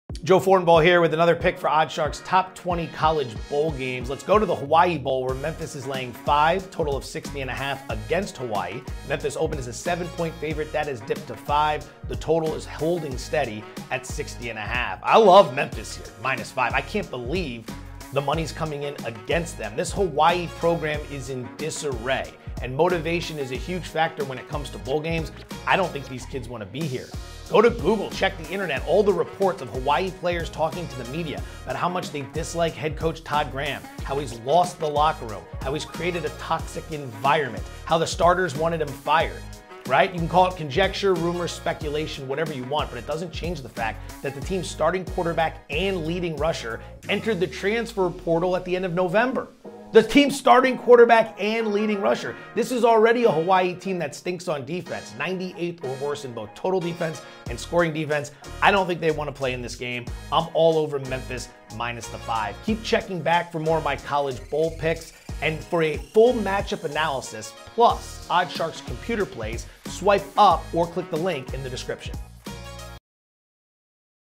Memphis vs Hawaii, Hawaii Bowl, Best Bet, Analysis & Prediction
Memphis enters the 2021 Hawaii Bowl 0-9 against the spread in its last nine games away from home. Can the Tigers reverse the trend against Hawaii?

Check out Joe Fortenbaugh's preview for the Rainbow Warriors vs Tigers bowl game set for December 24!